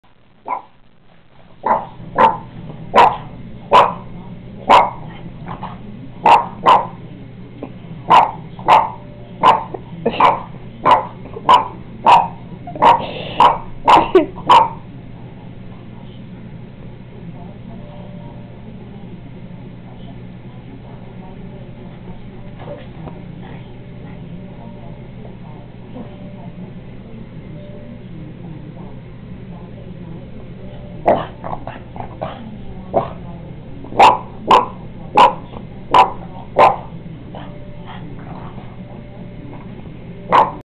My Boston Terrier Anna Does NOT Like This Painting.avi
As smart as this pup is, she sure looks silly barking at a painting of her beloved Otto as a puppy. Needless to say, I can't hang it up.